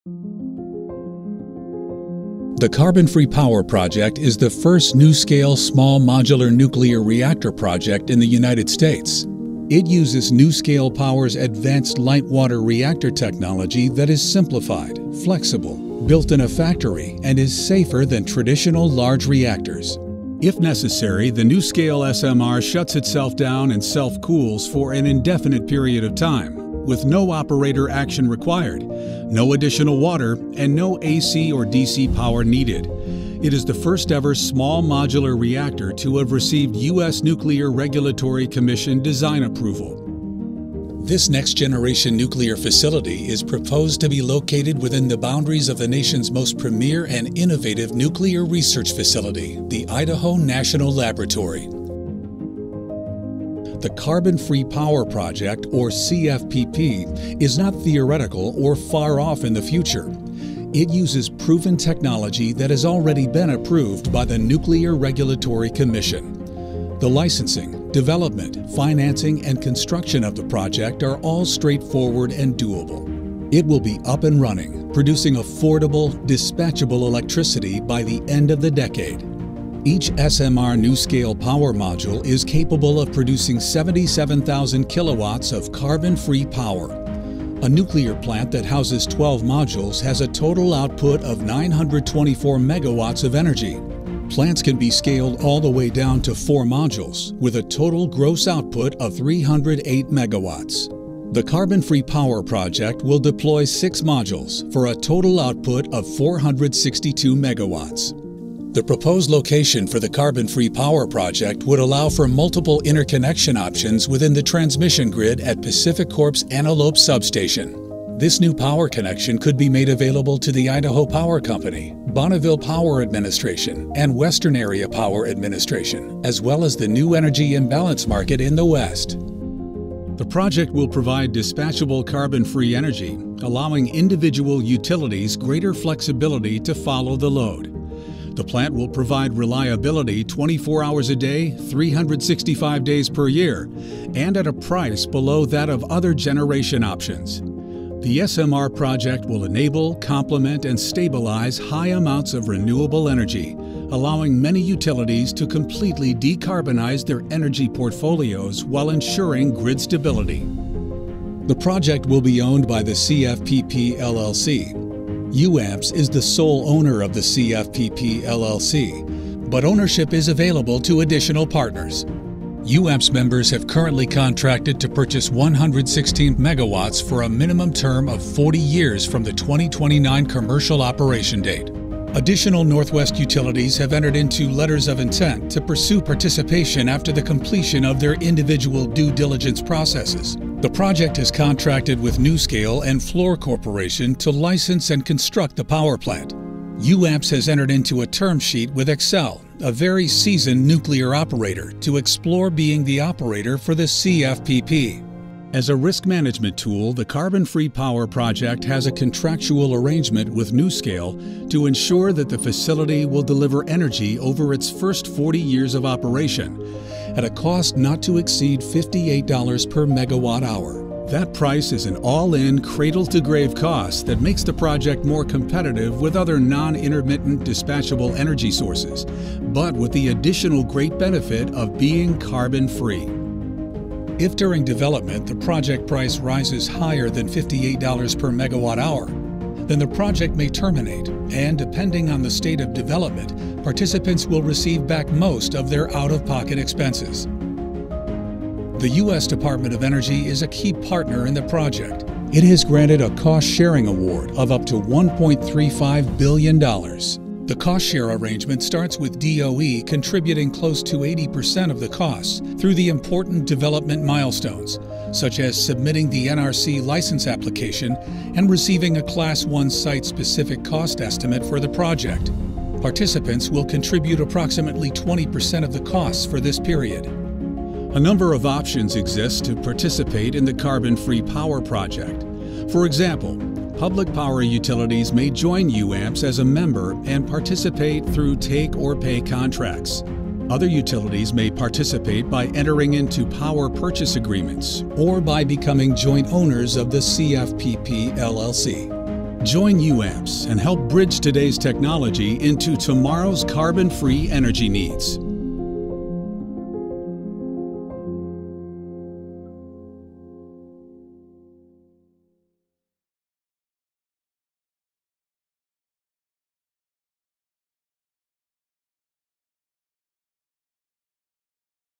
The Carbon Free Power Project (CFPP)
The Carbon Free Power Project (CFPP) will be the first NuScale Power small modular reactor (SMR) plant to begin operation in the United States near Idaho Falls, Idaho, at the DOE’s Idaho National Laboratory. The SMR plant will deploy six, 77-megawatt modules to generate 462 megawatts of carbon free electricity.